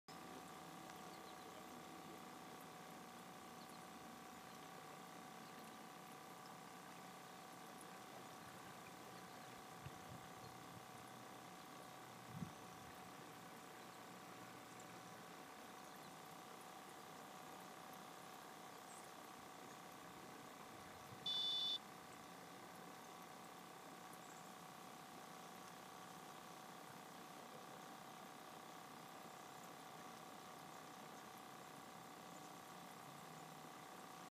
Remote control fishing boat
One pound bass. caught by this remote control boat.